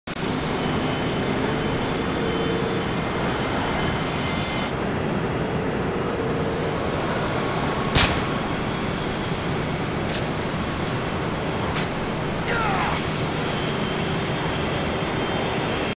Parker The Epic - Most Confusing Kill Cam I Have Ever Gotten.
Game Clip of a Private Match.

Come Game With Me: XBL: Parker The Epic

Feel free to send me a Friend Request or a Message.